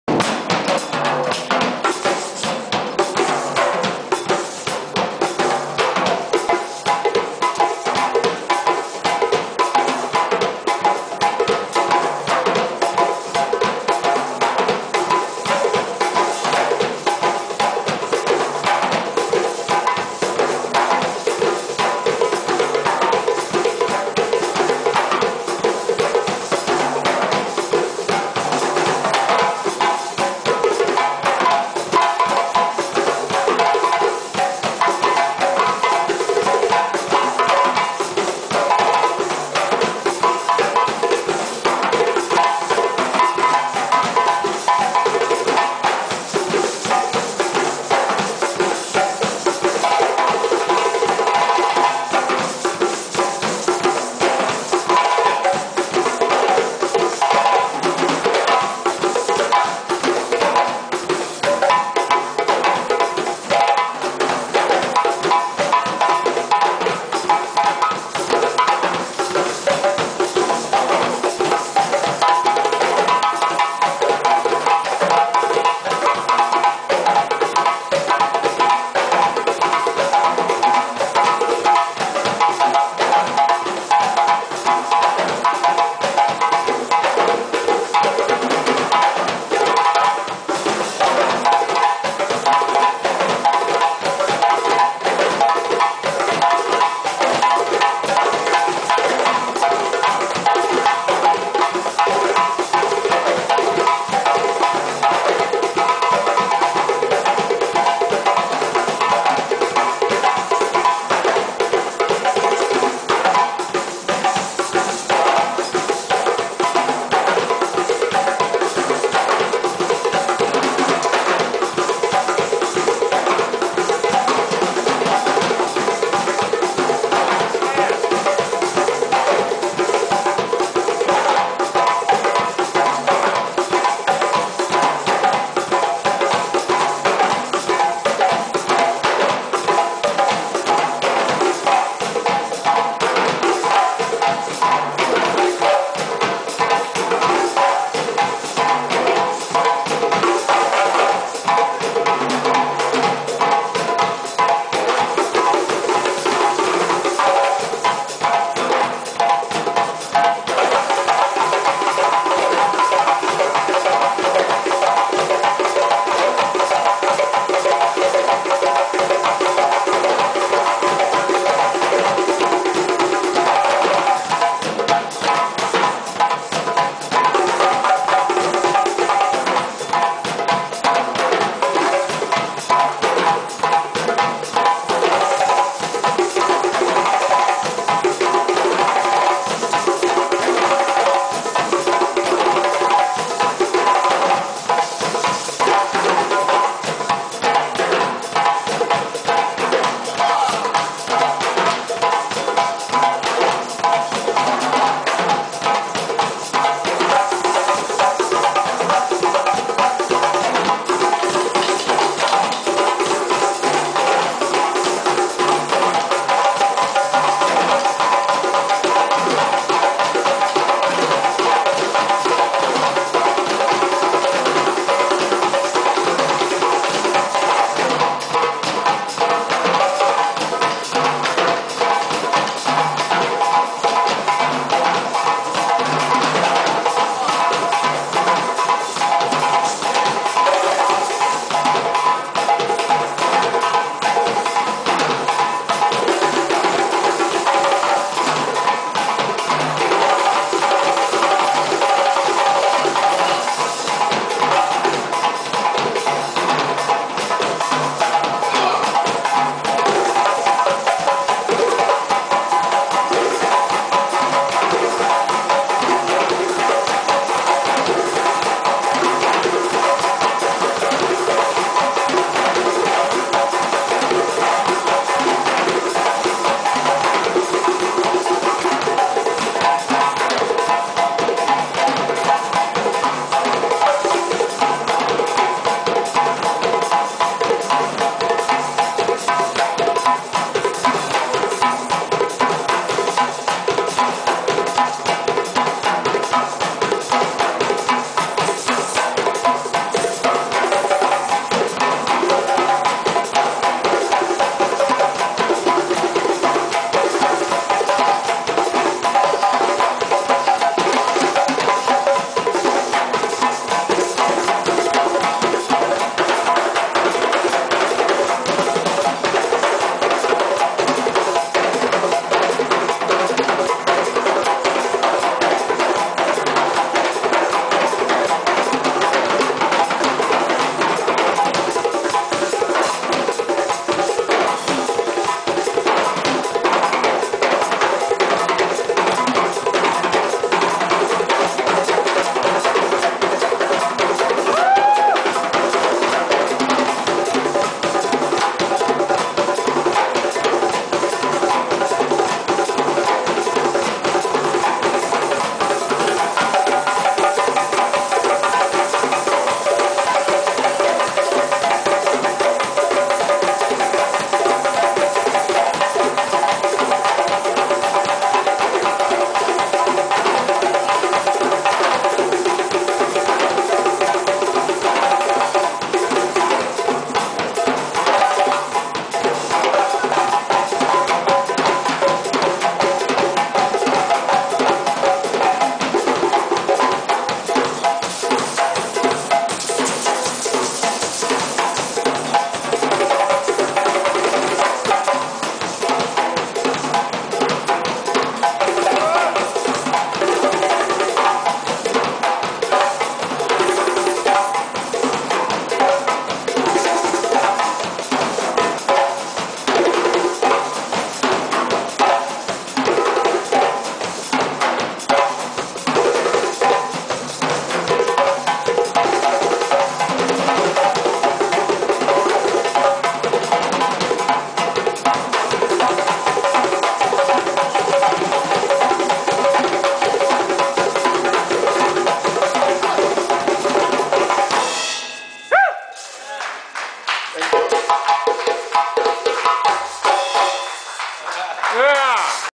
Seguenon Kone, Boyanna Trayanova and Mitch Caponetto Part 3 MOV07334.MPG
Seguenon Kone, Boyanna Trayanova and Mitch Caponetto perform on djembe and drum kit at the "Open Ear Drums" session, Jan. 10, 2012.  Open Ears Music Series, The event was organized and video'd by Dennis Formento.  Jeff Albert curates the series in the upstairs "balcony club" of the Blue Nile Music Club, 532 Frenchmen St., New Orleans.